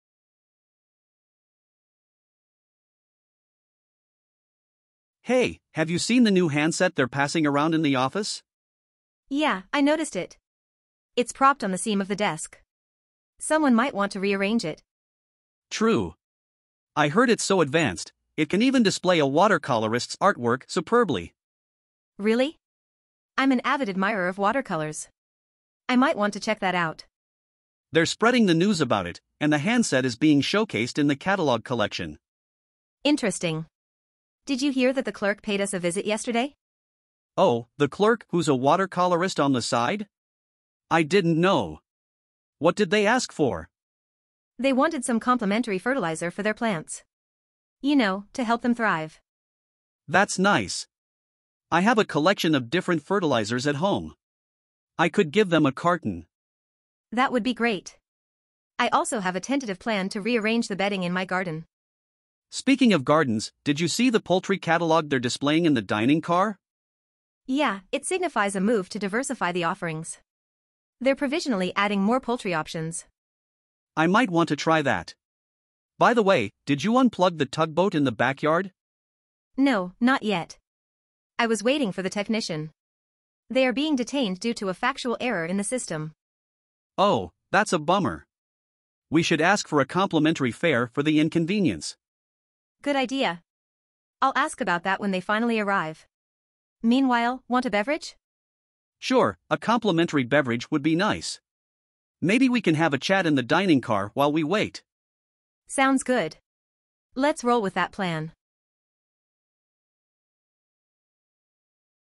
【Learn TOEIC vocabulary with conversation】Conversations and Catalogs
The meanings of each word in this conversation are here.

On the seam: Positioned along the junction or line where two surfaces meet.

Handset: A mobile or landline phone's receiver or a portable device used for communication.

Propped: Supported or held in place, often temporarily, by a prop or support.

Fertilizer: Substances added to soil to enhance plant growth and productivity.

Collection: A group of items gathered or assembled, often with a common theme or purpose.

Might want to: Expressing a suggestion or possibility, indicating a potential desire or action.

Succeeding: Following in sequence or coming after; the next in line.

Carton: A box made of cardboard or other material used for packaging and transporting goods.

Displaying: Presenting or exhibiting something for others to see.

Unplug: Disconnecting an electrical device by removing its plug from a socket.

Rearrange: Change the order, position, or layout of something.

Bedding: Materials like sheets and blankets used for covering a bed.

Roll: To move by turning over and over, or a list or group of items presented in sequence.

Tugboat: A powerful boat used for towing larger vessels, especially in harbors.

Spreading: Extending or distributing over an area.

Paid us a visit: Came to see or spend time with someone.

Tentative: Not certain or fixed; done without confidence; hesitant.

Ask for: Request or inquire about something from someone.

Complimentary: Provided free of charge or as a gesture of goodwill.

Fare: The cost of transportation, especially for a journey on public transport.

Dining car: A compartment on a train where meals are served to passengers.

Signify: To convey or indicate meaning or importance.

Detain: Keep someone in official custody, typically for questioning or investigation.

Thrive: Grow or develop well; prosper; flourish.

Watercolorist: An artist who specializes in creating paintings using watercolors.

Admirer: Someone who has a favorable opinion or appreciation for someone or something.

Superbly: In an excellent or outstanding manner.

Avid: Having a strong passion or enthusiasm for something.

Poultry: Domestic birds kept for their eggs and meat, such as chickens or ducks.

Provisionally: Temporarily or subject to future confirmation.

Diversify: Introduce variety or a wider range of elements.

Factual error: Mistake or inaccuracy based on facts or reality.

I hope this video is useful for your learning English.
Use it for listening, shadowing, and reading!